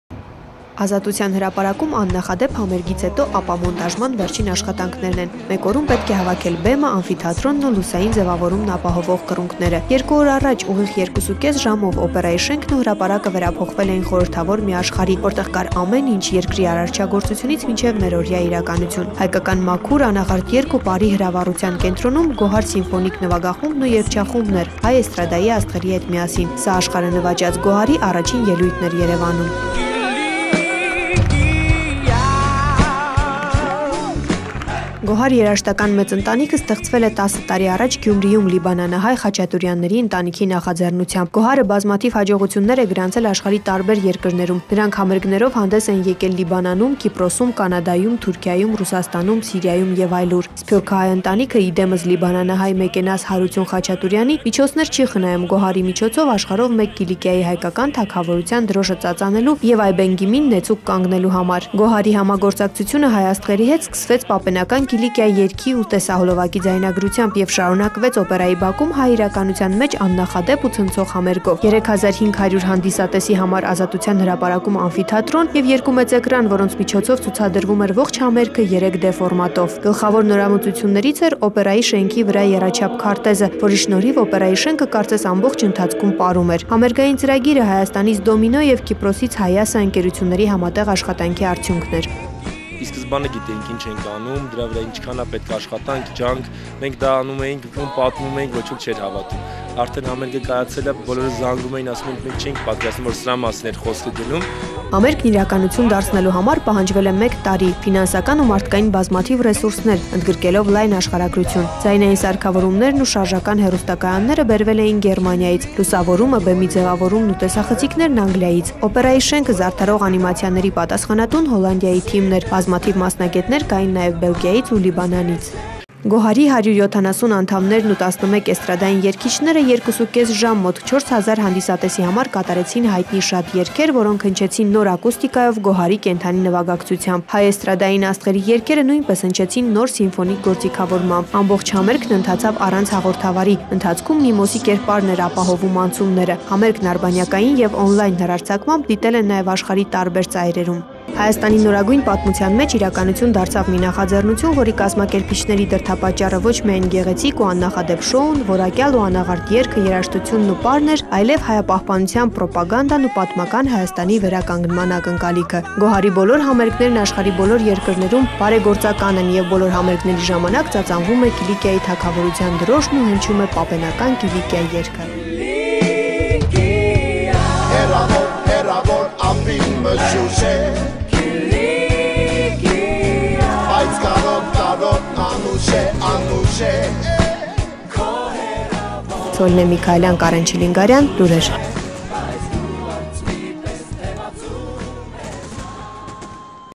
TV Coverage on DOMINO Production work at KOHAR Concert in Yerevan 2011
For the realization of the concert 14 trucks of concert equipment, 2 mobile production vans from Belgium and an enormous number of sound-equipment from Germany had been shipped. Also, more than 150 professionals from Armenia and 8 invited companies from abroad with their 60 professionals had been involved.

In the frames of the concert, for the first time in the region, a 5-minute 3D animation show (3D mapping) was implemented in the building of the National Academic Theatre of Opera and Ballet. We find it necessary to point out that 3D animation show was an innovation on the semi-circular building and was carried out for the first time.